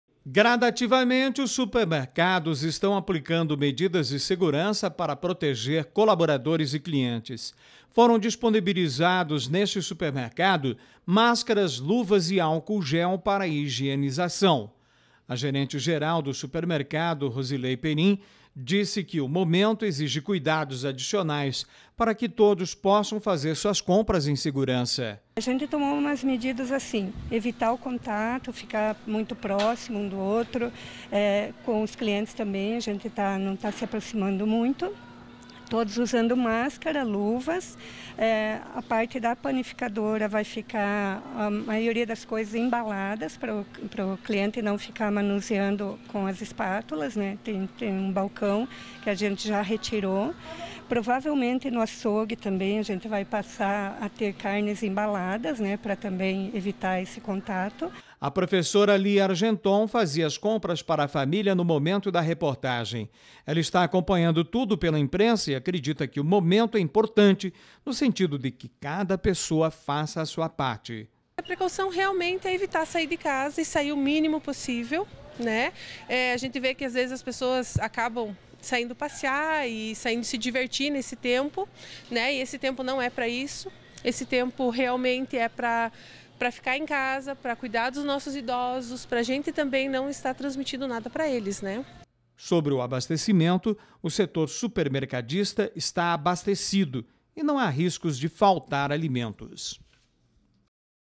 Supermercados adotam medidas para proteger colaboradores e clientes nestes tempos de Pandemia
Imagens: Paulo Tessaro.